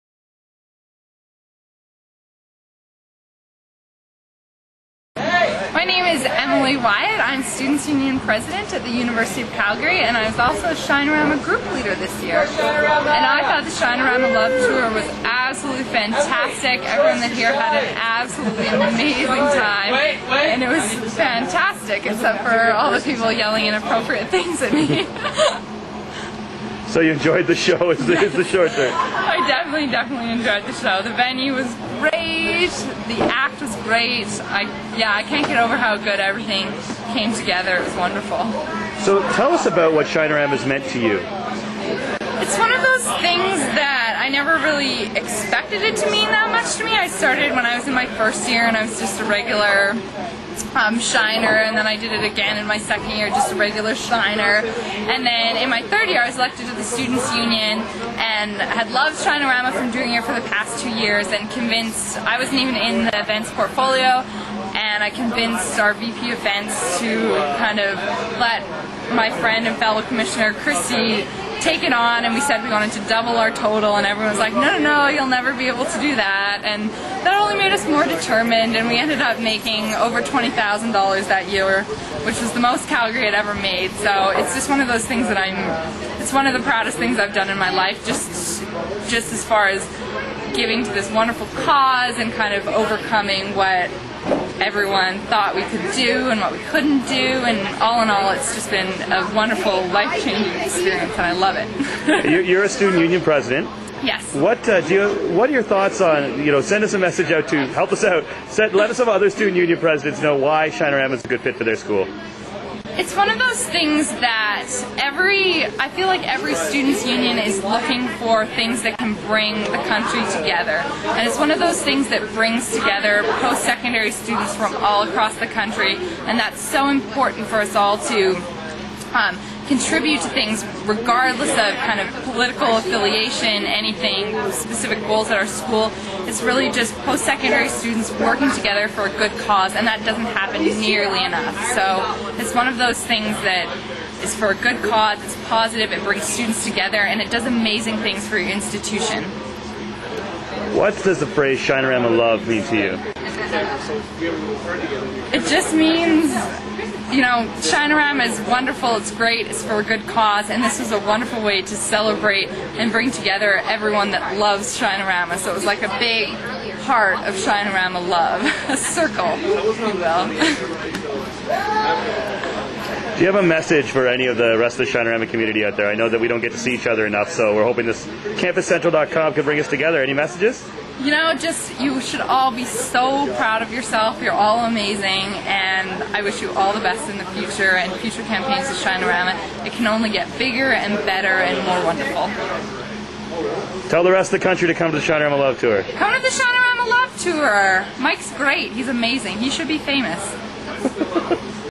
An Interview with UC Student Union President Emily Wyatt
Shinerama Group Leader and University of Calgary Student Union President Emily Wyatt talks about why Shinerama is such a good fit for University and College student governments.